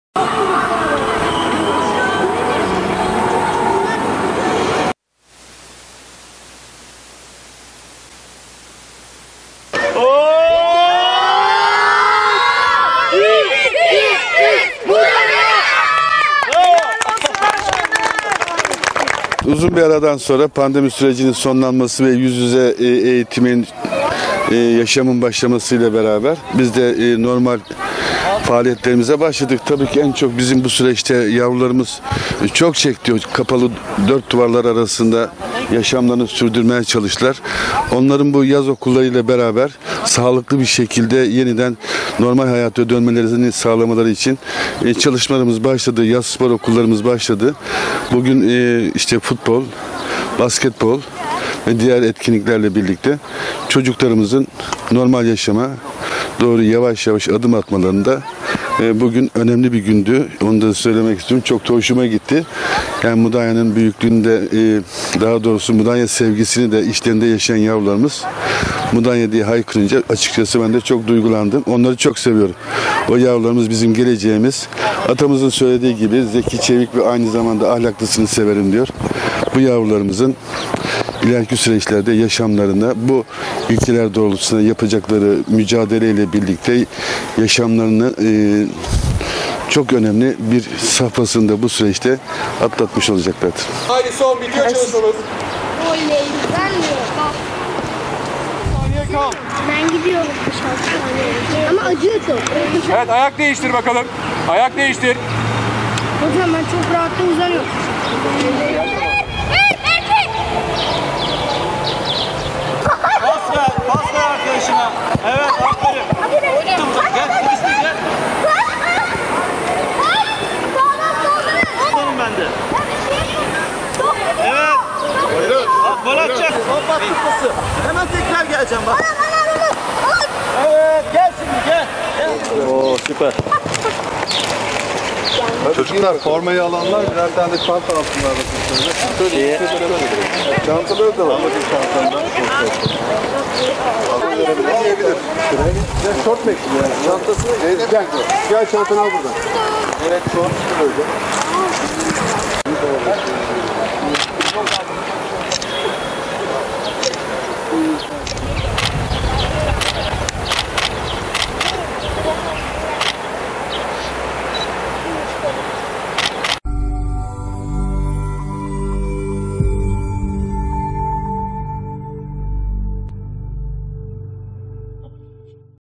YAZ SPOR OKULU’NDA FORMALAR BAŞKAN TÜRKYILMAZ’DAN
Mudanya Belediyesi’nin düzenlediği yaz spor okulları sürüyor.  7-11 yaş arası çocuklar için futbol ve basketbol branşlarında düzenlenen kurslarda, çocuklar hem eğleniyor hem de pandemi nedeniyle uzun süredir hasret kaldıkları etkinliklerin tadını çıkarıyor.
Genç sporcular, profesyonel antrenörler eşliğinde temel futbol eğitimleri alıyor. Futbol Yaz Spor Okulu’nu ziyaret eden Başkan Hayri Türkyılmaz, çocuklara forma ve çanta dağıttı.
Ücretsiz olarak düzenlenen Yaz Spor Okulları ile Mudanyalı çocukların spor yapmalarına, becerilerini geliştirmelerine olanak sağladıklarını kaydeden Başkan Hayri Türkyılmaz, “Pandemi döneminde en çok dört duvar arasında kalan çocuklarımız etkilendi. Yaz spor okulları ile birlikte sağlıklı bir şekilde yeniden normal hayata dönmeleri için çalışmalarımızı başlattık. Yaz tatilini verimli bir şekilde geçirmelerini amaçlıyoruz. Amacımız öğrencilerimizi geleceğe en iyi şekilde en güvendiği ortamda hazırlamak. Eğitimi, sporu, kültür ve sanatı herkes için ulaşılabilir kılan belediyemizin çalışmaları sürecek. Geleceğimiz, yarınlarımız çocuklarımız için ne yapsak az” dedi.